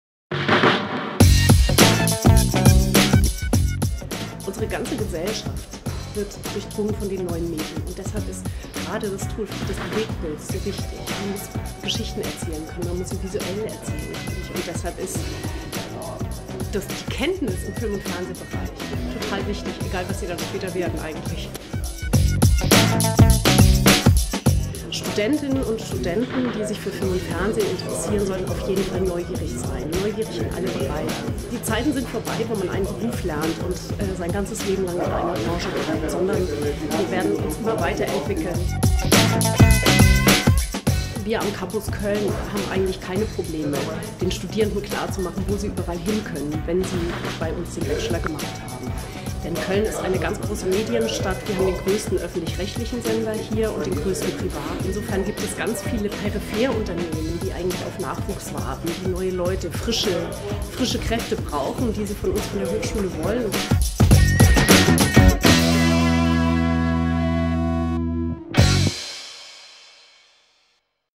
Film studieren an der Hochschule Macromedia
Welche Kompetenzen vermittelt das Filmstudium (B.A.)? Für welche Berufe qualifizieren sich die Studierenden, welches sind typische Arbeitgeber?

Das erläutert Professor Sibylle Stürmer. Sie unterrichtet Film am Campus Köln der Hochschule Macromedia.